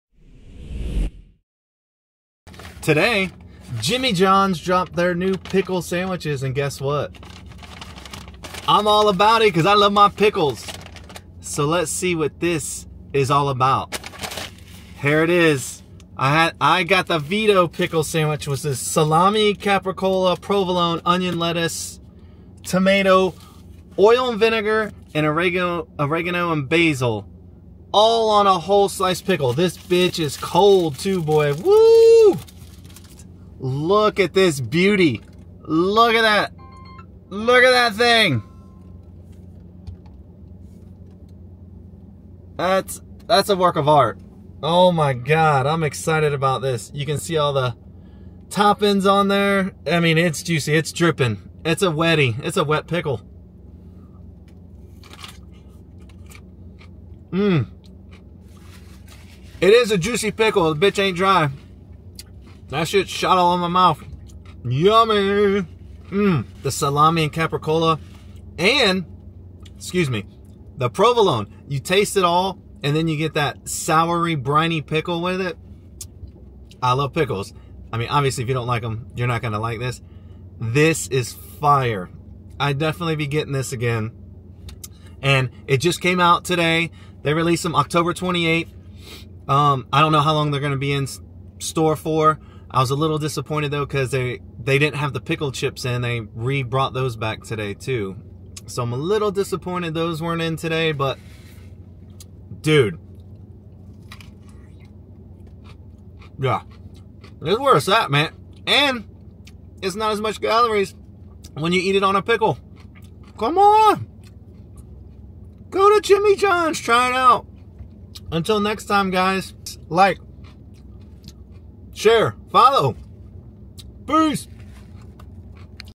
Trying Jimmy Johns New Pickle Sandwich the Vito Picklewich!!!!🔥🔥🥒🥒🥒🔥🔥🔥
Today Jimmy John's release their new pickle sandwiches and I am trying the new Vito Picklewich. This beautiful pickle sandwich includes Salami,Capocollo,Provolone,Onion,Lettuce,Tomato,Oil and vinegar and oregano-basil seasoning in a whole sliced juicy pickle. Let's see how good these pickle sandwiches are. LFG!!! 🥒🥒🥒🥒😋😋😋